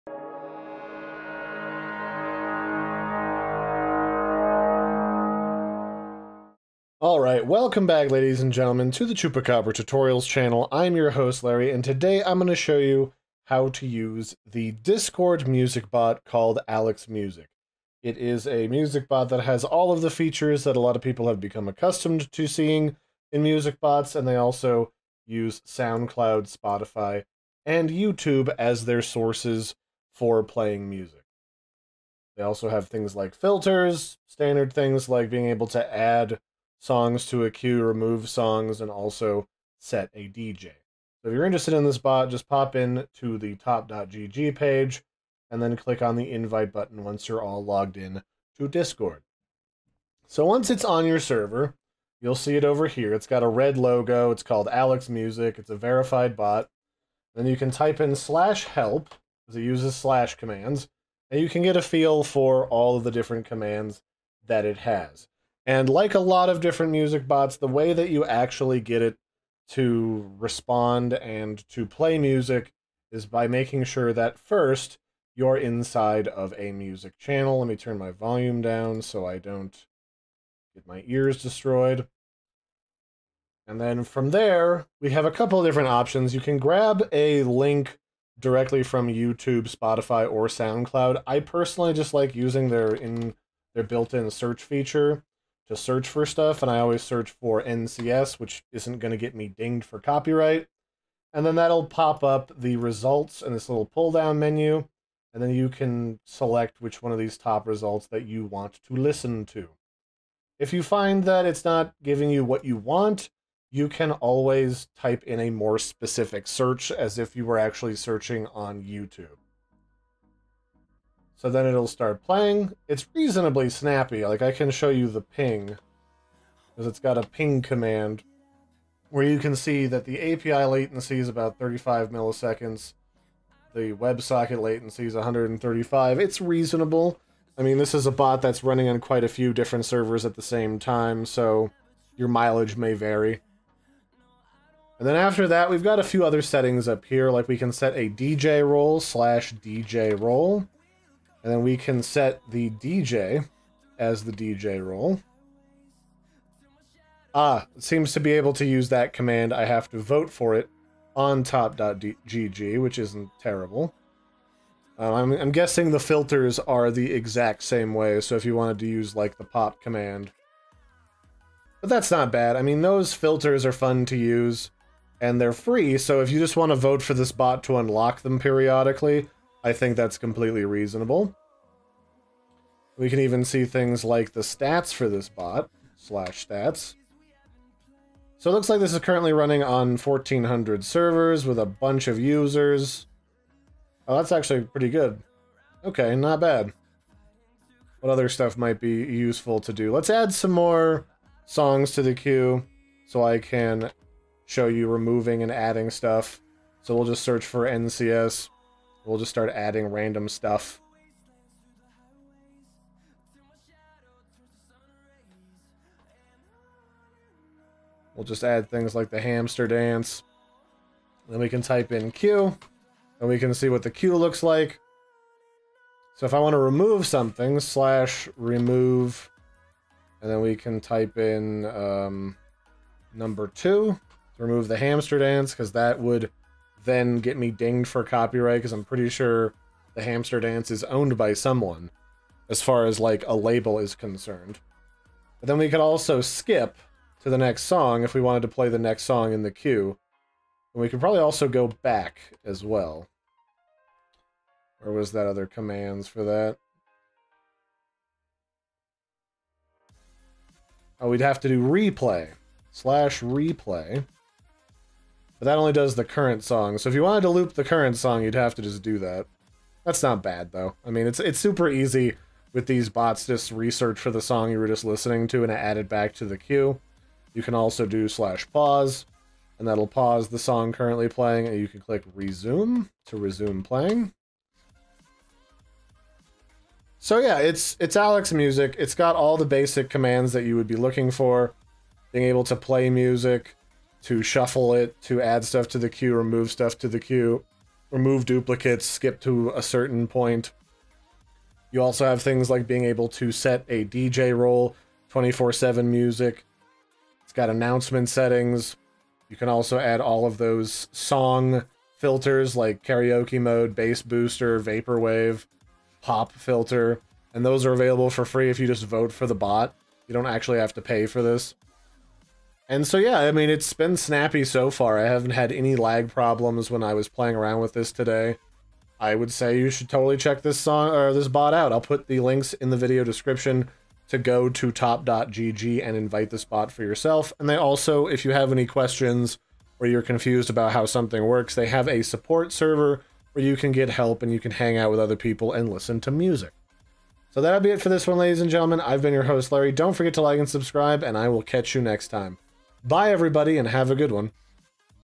Alex Music Bot 2022 Setup Guide - Play Music from Youtube, Spotify, & Soundcloud
Quick guide on how to invite, setup, and use Alex Music bot to play music on your discord server. Supports playing music from Soundcloud, spotify, and Youtube. Includes all the most common music bot commands, ability to set a DJ, and more!